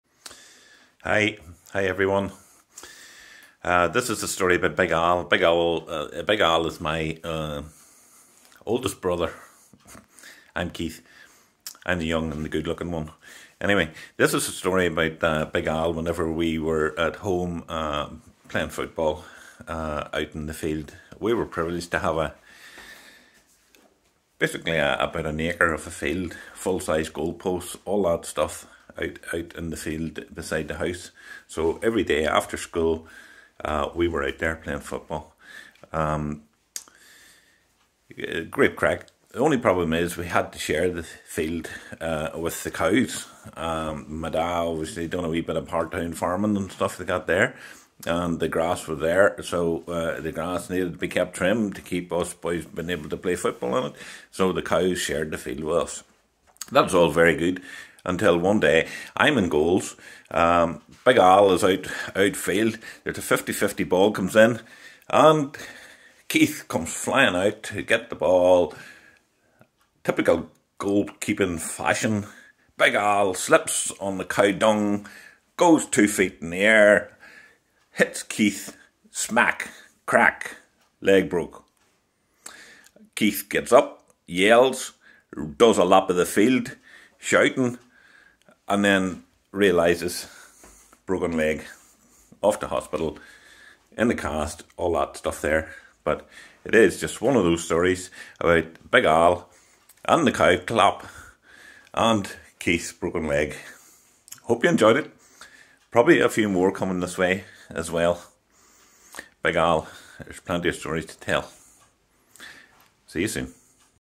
Keith Gibson: "Cow Clap". 65th fundraiser anecdote contribution.
Thanks to young bro Keith for telling this Big Al Story. Yes our family also had those nasty teenager football memories!

I encourage everyone to please donate to The Trussell Trust fund helping the work of UK Foodbanks - part of Big Al's 65th Creative Stories Fundraiser.